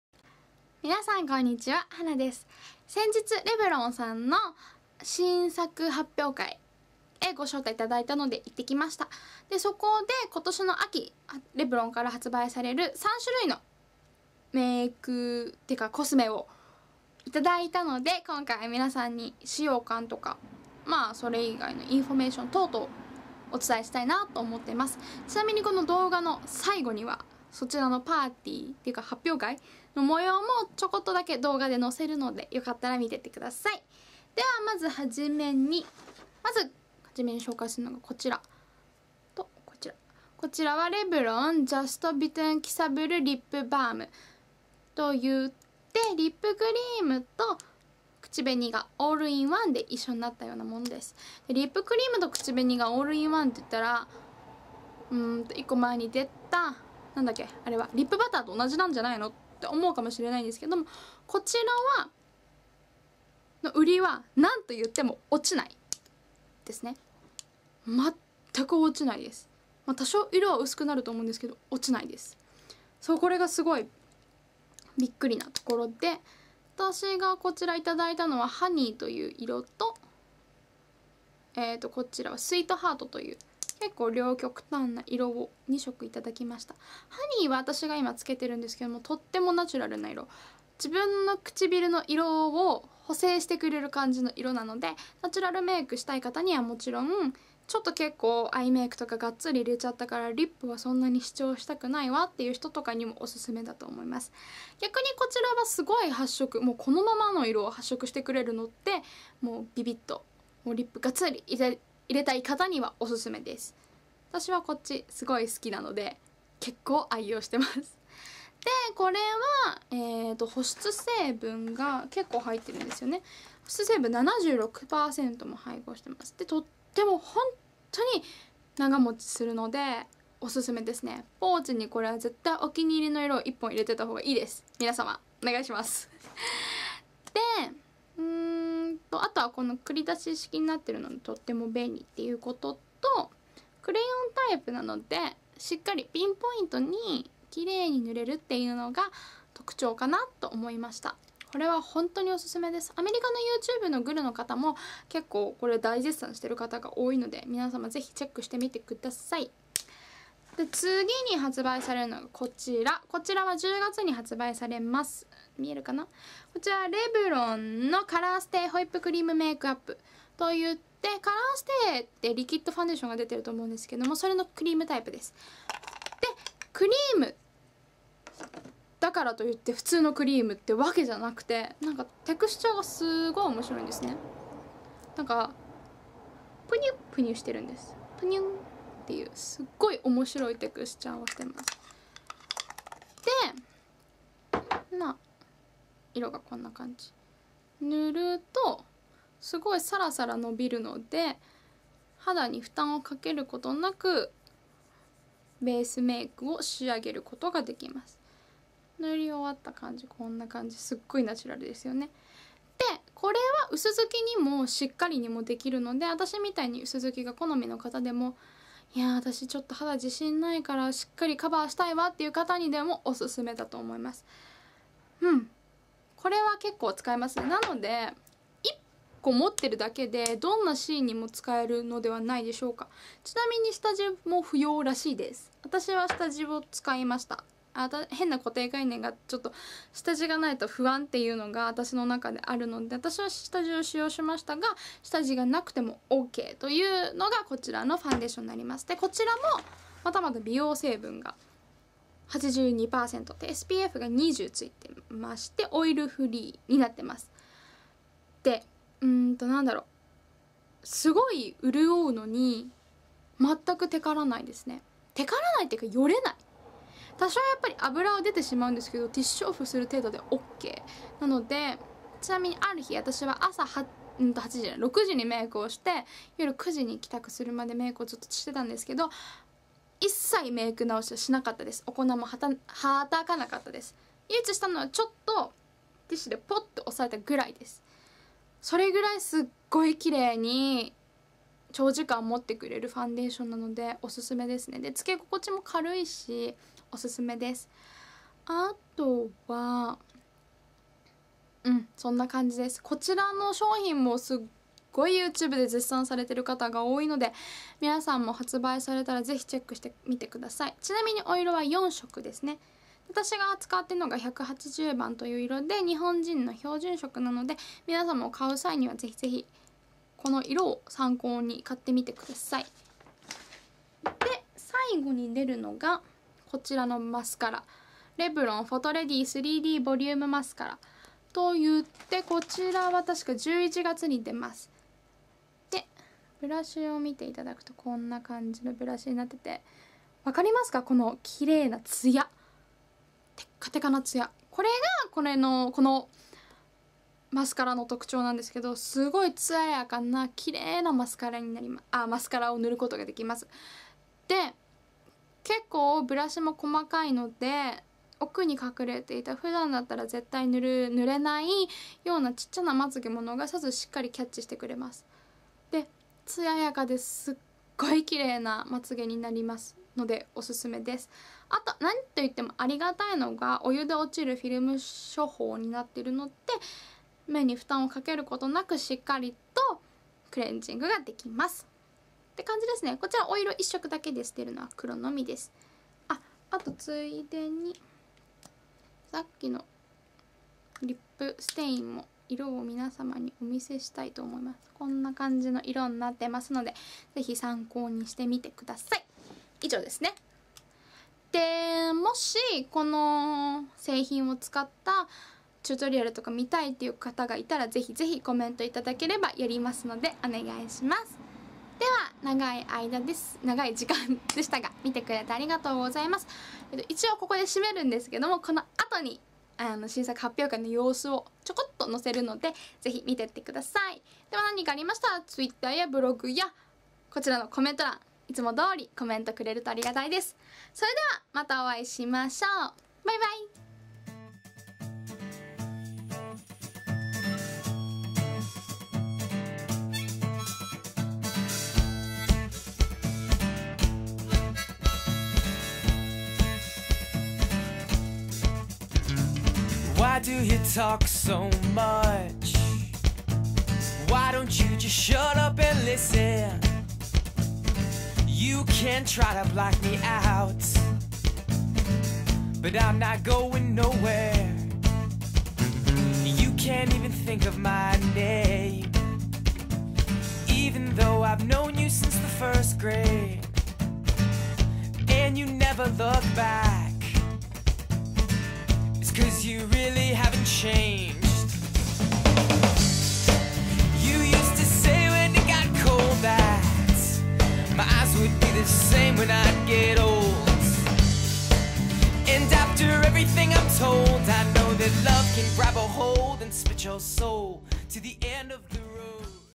REVLON NEW PRODUCTS REVIEW/ PREMIER
Products mentioned:
REVLON:Just Bitten Kissable Balm Stain
REVLON:ColorStay Whipped Creme Makeup
REVLON:PhotoReady 3D Volume Mascara

CHECK MY NEW ENGLISH BLOG!!!